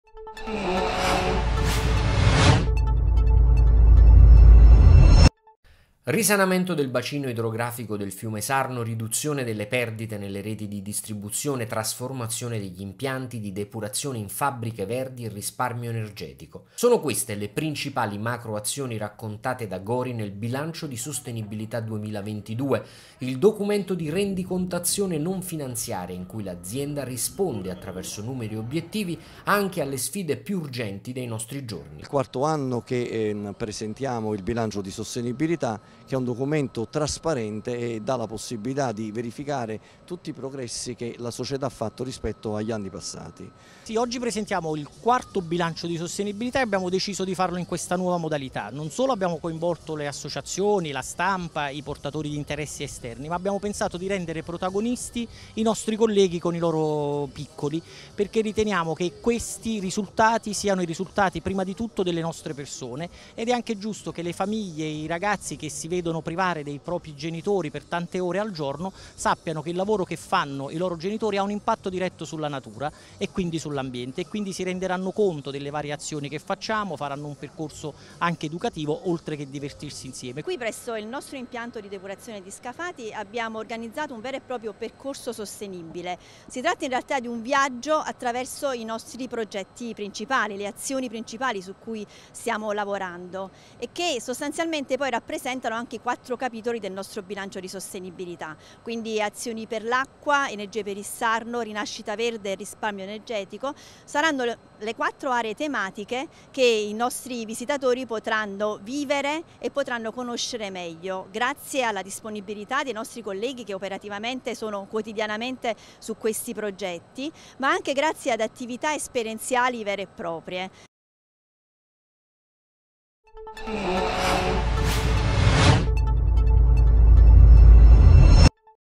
Gori presenta il bilancio di sostenibilità: progetti avviati e obiettivi raggiunti
Risanamento del bacino idrografico del fiume Sarno, riduzione delle perdite nelle reti di distribuzione, trasformazione degli impianti di depurazione in fabbriche verdi e risparmio energetico. Sono queste le principali macro-azioni raccontate da Gori nel Bilancio di Sostenibilità 2022: il documento di rendicontazione non finanziaria in cui l’azienda risponde attraverso numeri e obiettivi anche alle sfide più urgenti dei nostri giorni. La presentazione delle attività e dei risultati conseguiti in materia ambientale, sociale ed economica è avvenuta ieri pomeriggio a Scafati, presso l’impianto di depurazione gestito da Gori, in una modalità nuova e interattiva che ha previsto l’allestimento di un percorso espositivo tematico dedicato ai principali progetti su cui l’azienda è al lavoro. Ed è proprio nell’ottica della partecipazione che l’evento ha visto il coinvolgimento delle persone di Gori, delle associazioni, e, soprattutto, di tantissimi bambini in età scolare, che, attraverso attività ludico-formative, hanno appreso numerose buone pratiche per garantire a tutti un futuro più sostenibile.  Un’altra occasione di dialogo con il territorio e di sensibilizzazione ambientale verso le nuove generazioni, a cui hanno partecipato il Presidente di Gori, Sabino De Blasi, l’Amministratore Delegato, Vittorio Cuciniello, la Responsabile Comunicazione e Corporate Social Responsibility, Mara De Donato, insieme agli “Ambasciatori della Sostenibilità” e a tutto il personale direttamente coinvolto nei progetti.